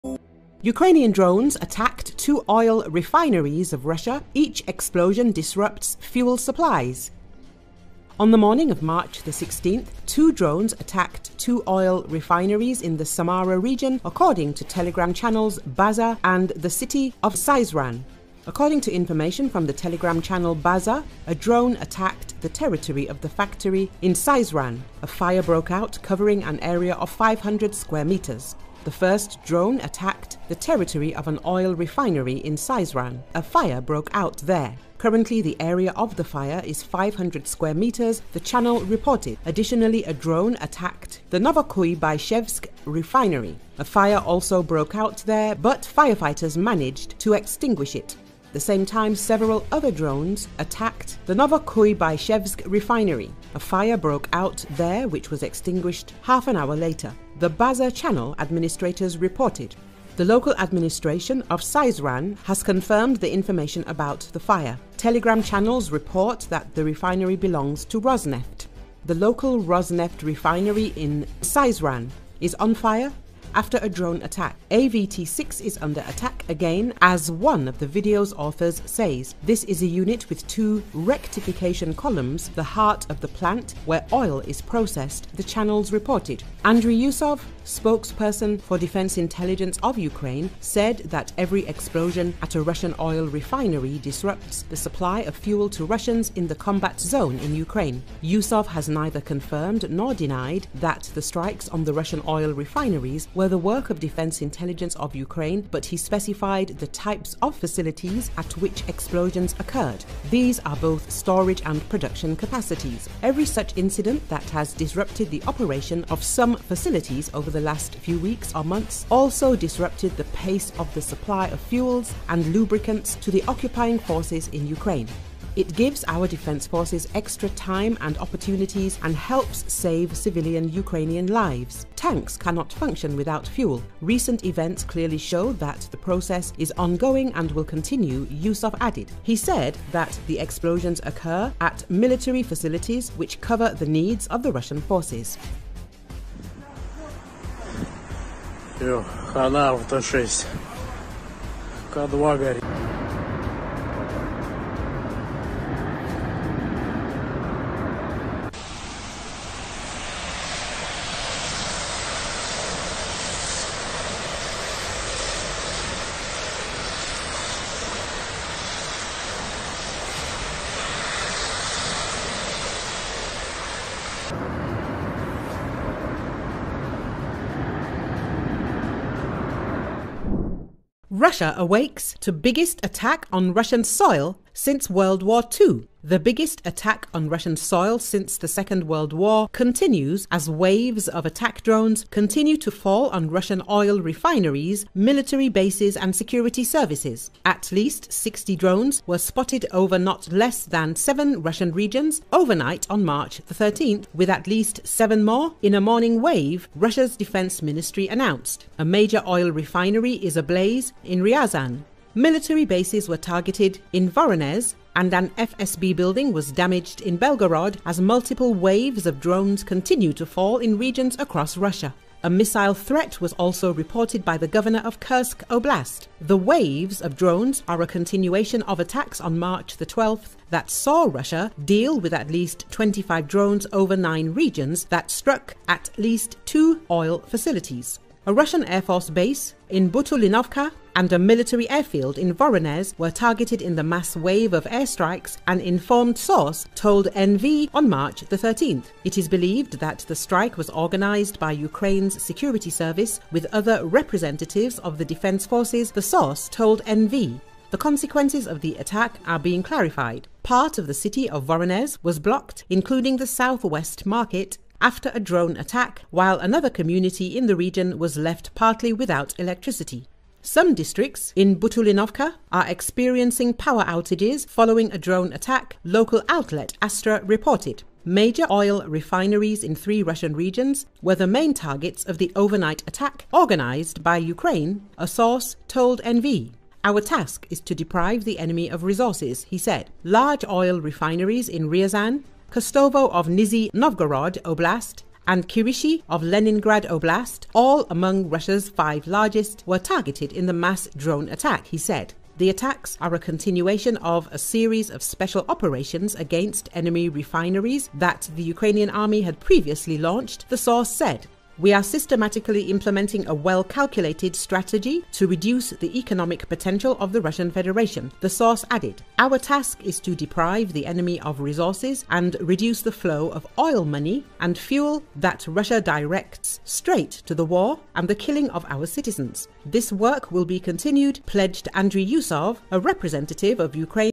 Russia under massive Drones attack
Russia under drones attack _Oil refineries disrupt fuel supplies for front